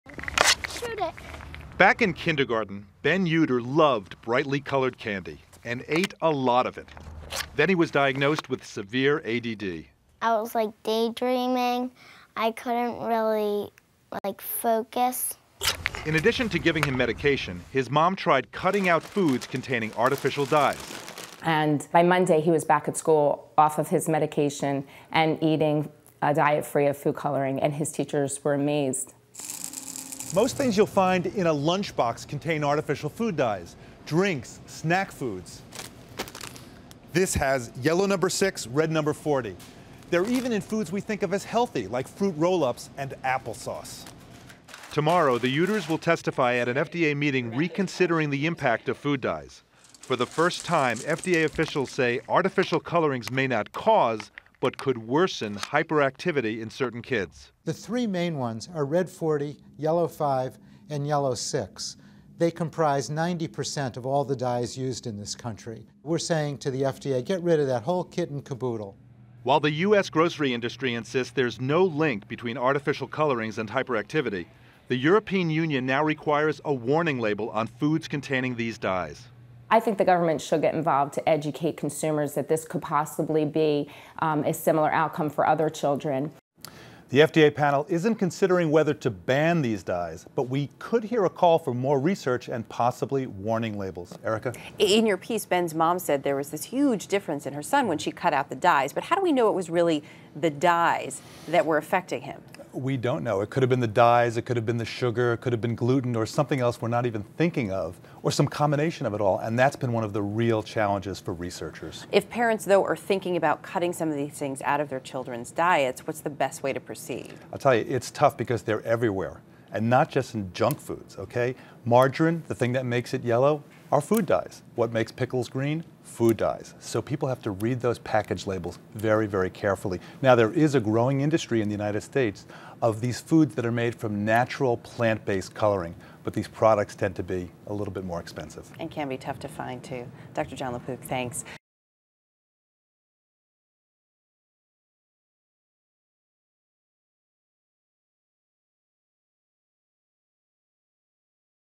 FDA looking into food dye, ADHD link
The Federal Drug Administration is reconsidering the impact of food dyes and its possible link to worsening ADHD in children. Dr. Jon LaPook reports.